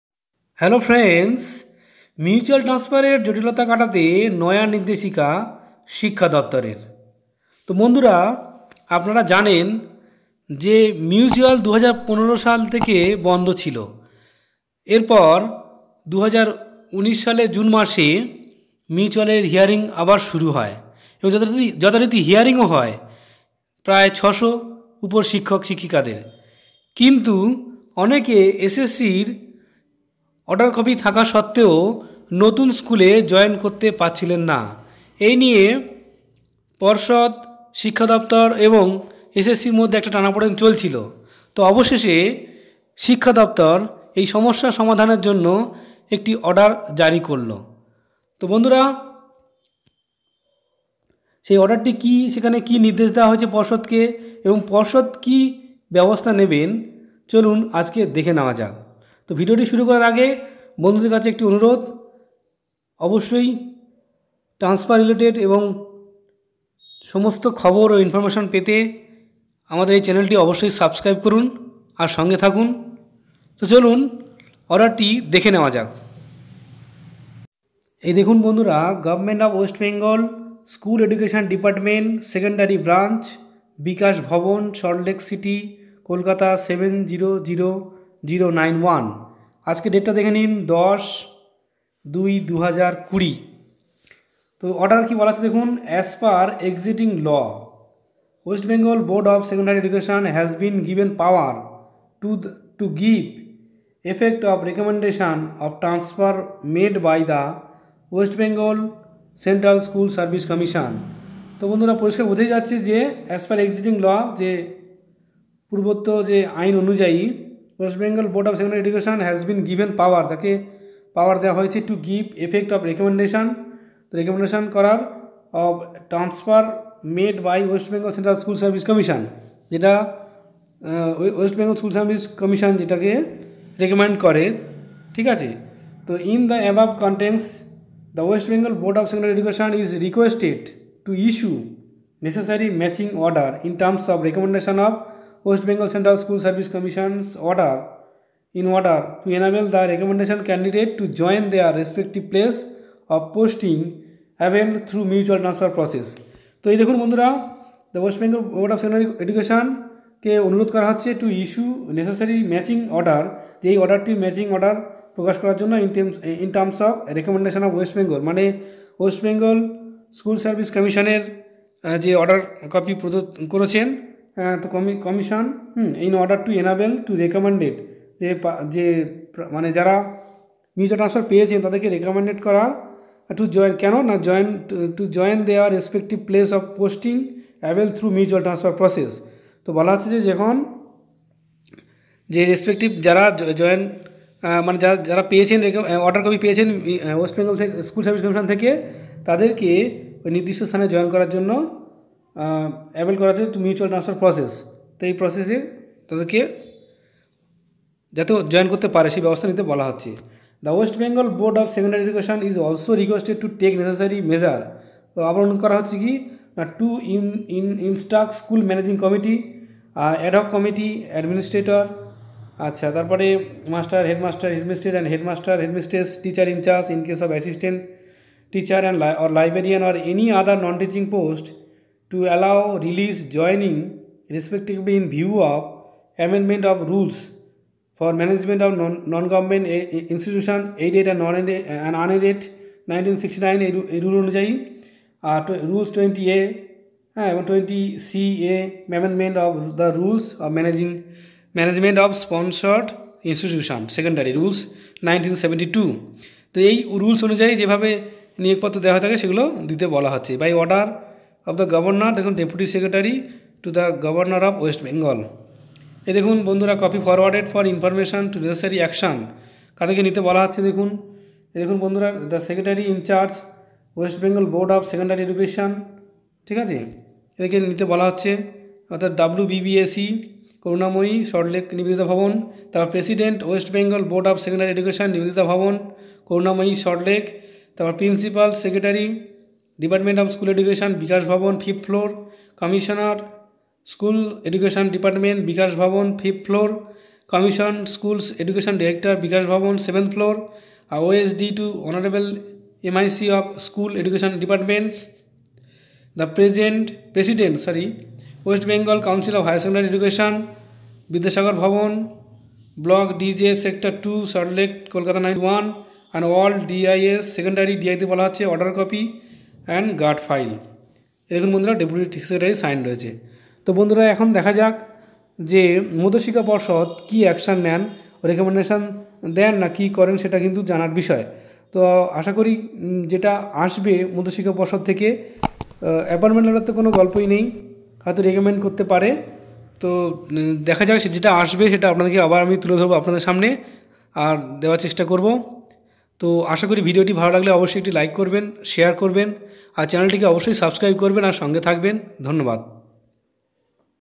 New order published about mutual transfer by education department
পর্ষদকে কি নির্দেশ দিল শিক্ষা দপ্তর
Appointment letter কি issue করবে wbbse?